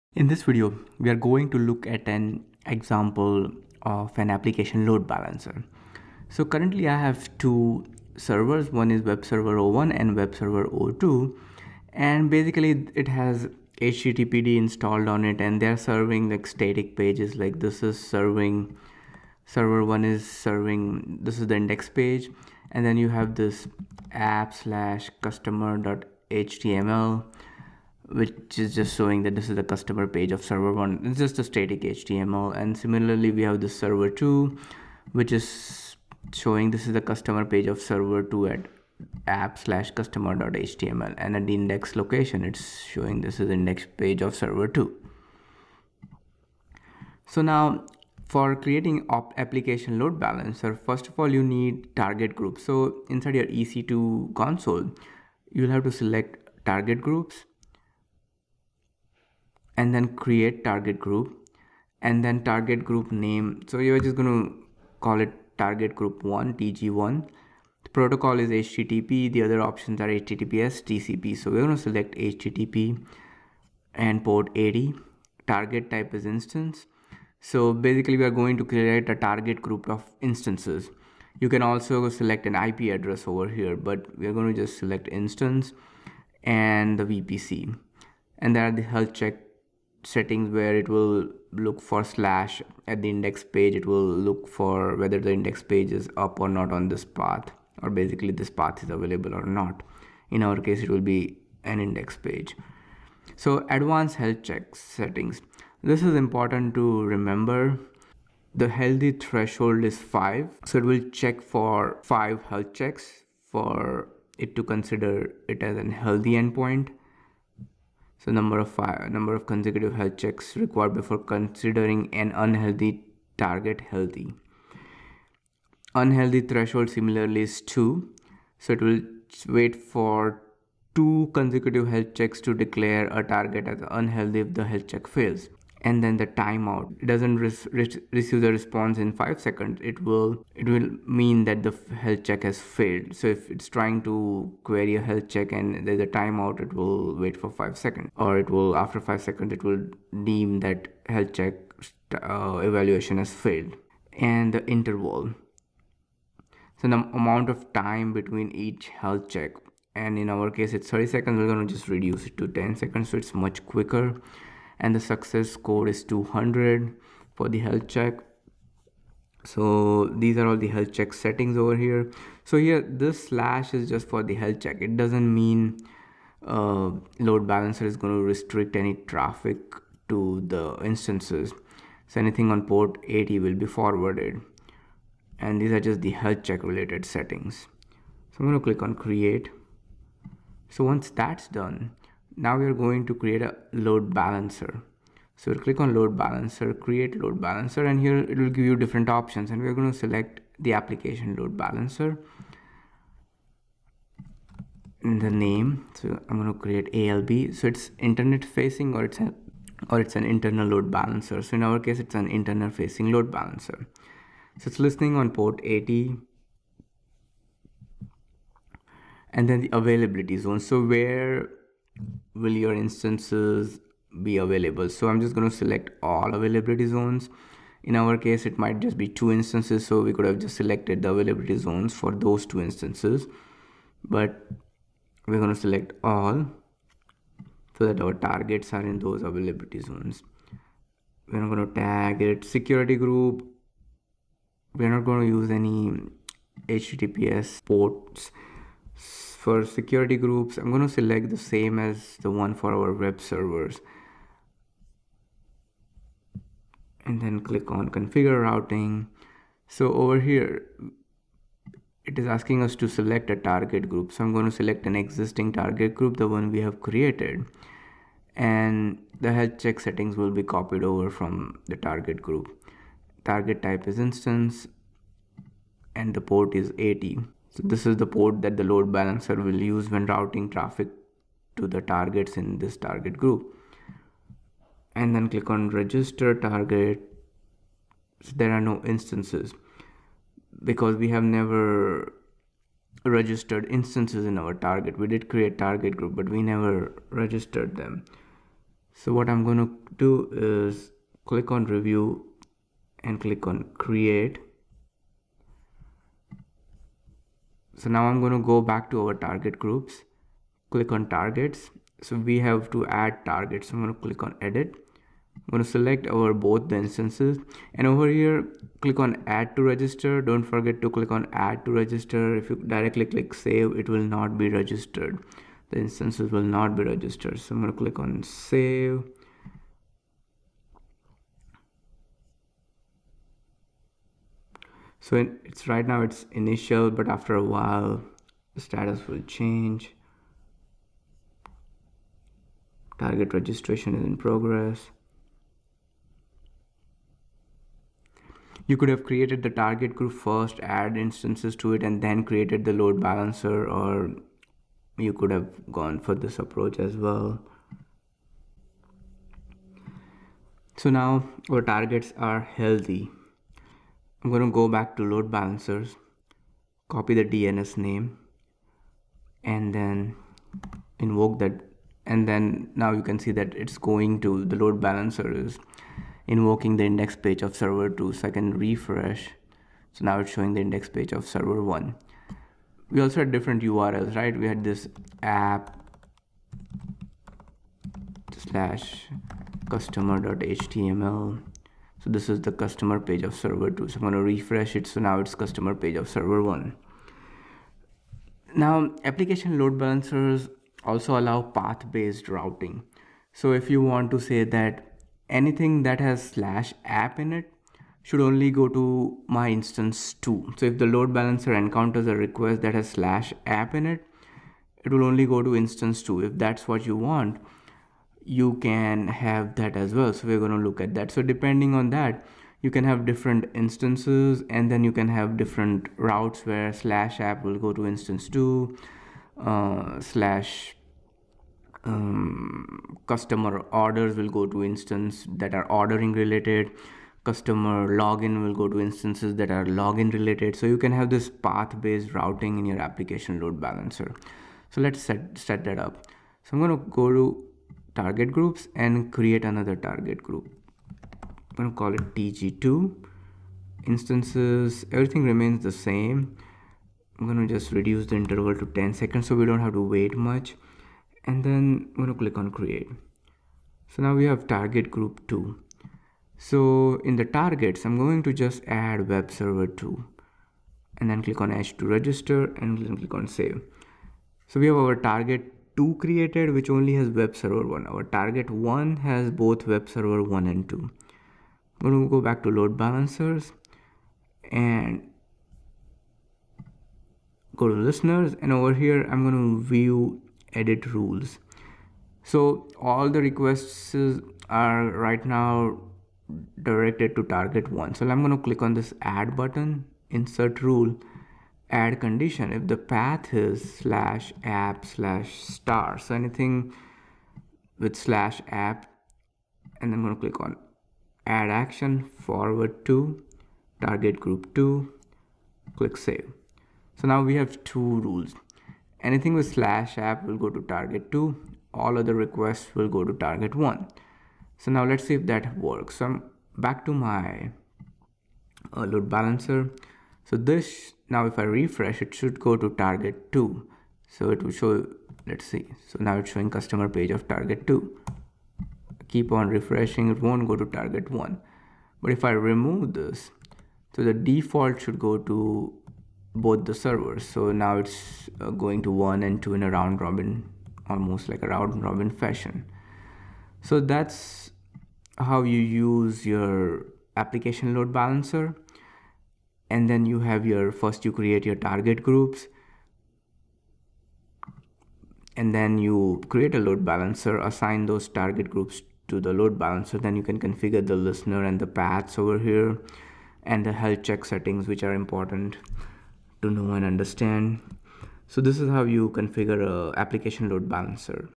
AWS Application Load Balancer
Application Load Balancer Demo
How to create Target Groups
How to create ALB forwarding rules.